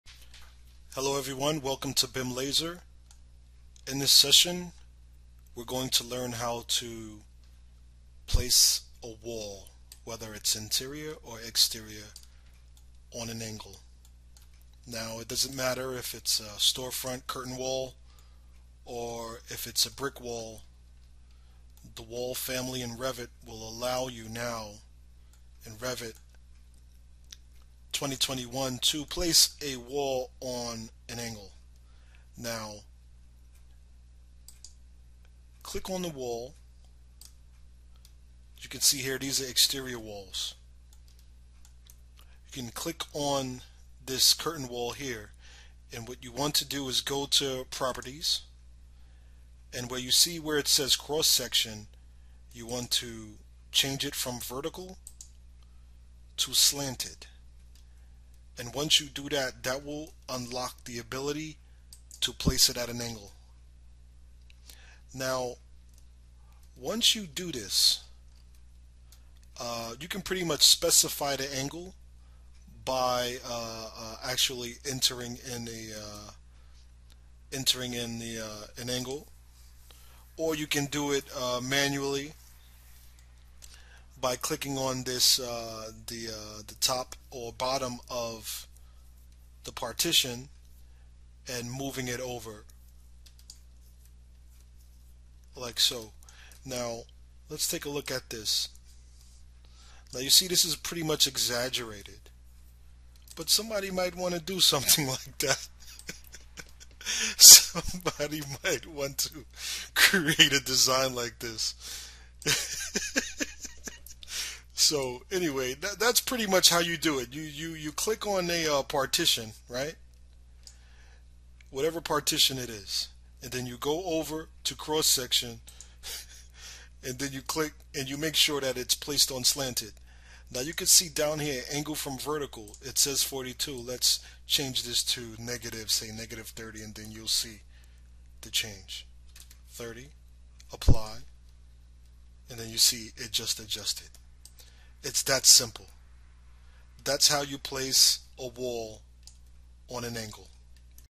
How to Model angled slanted walls in Revit 2021
In this video you will learn how to place a curtain wall on an angle in Revit 2021 with out using the massing technique

Amazon link to the HYPERTECT book

Orange Juice turtle necks, Shirts and hand made jeans